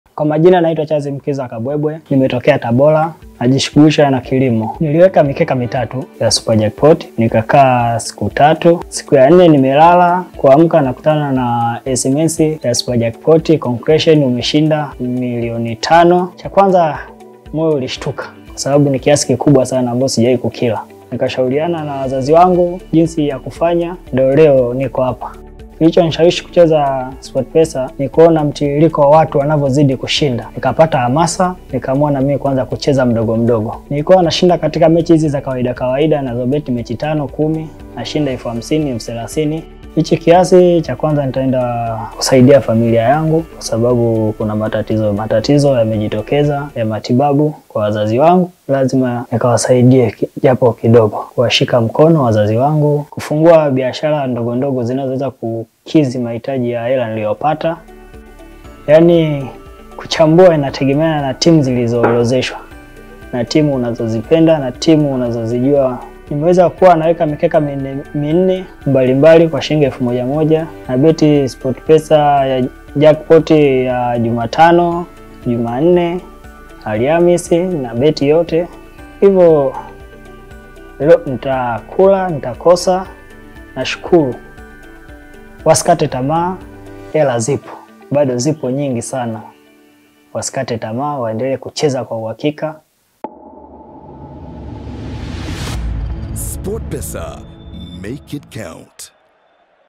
SportPesa Mshindi wa Mil 5,423,697 | Charles Kabwebwe
🎥 Kutana na mshindi wa SUPA JACKPOT BONUS 5,423,697/=⁣
✍🏻 Alipata bonus kwa kupata mechi 14 kati ya 17 kwenye mkeka wake⁣
🙏🏼CHARLES MUKIZA KABWEBWE⁣
📱 Bashiri mechi 17 za SupaJackpot kwa usahihi ushinde zaidi ya BILIONI kwa Buku tu⁣